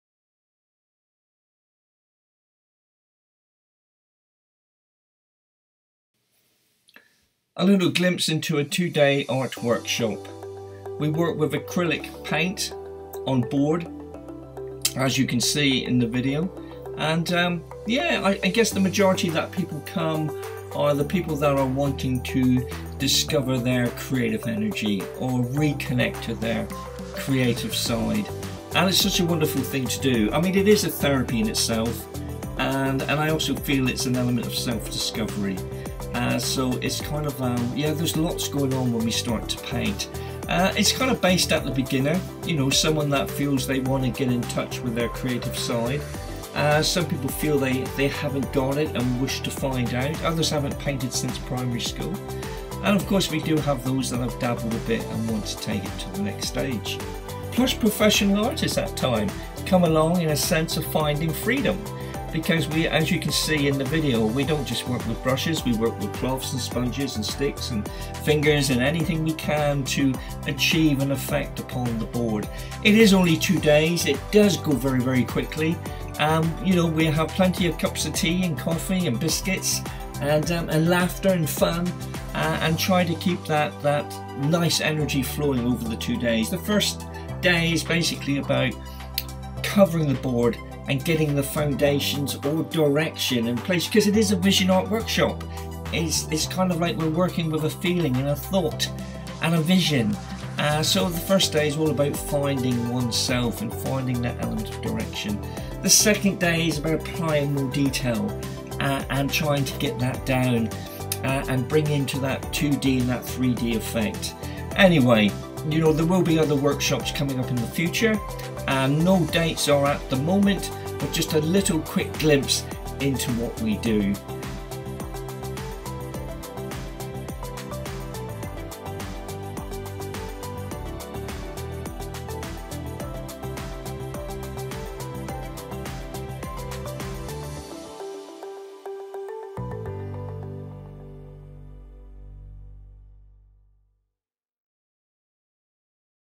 2 Day Vision Art Workshop
From the beginner to those that dabble, great therapy and self discovery in art.